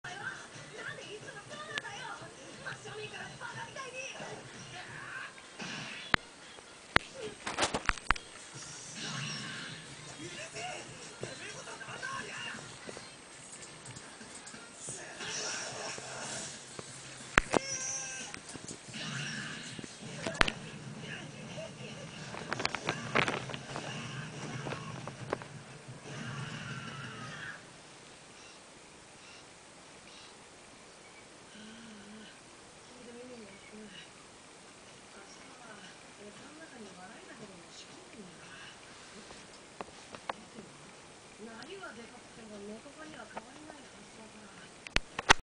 耳曲がり
今日は自分でカバーの中に・・・。
耳が曲がってる☆